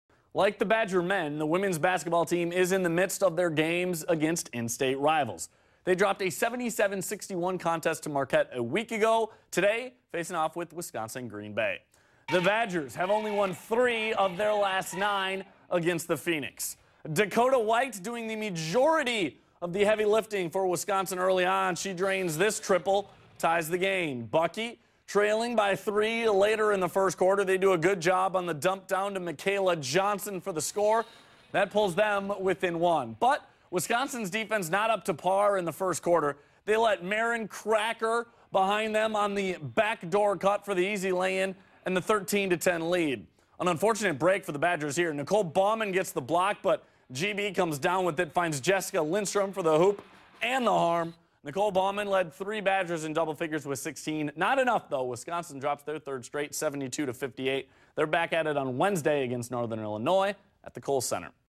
Badger women fall to Wisconsin-Green Bay
Wisconsin dropped their third straight game, this one a 72-58 final.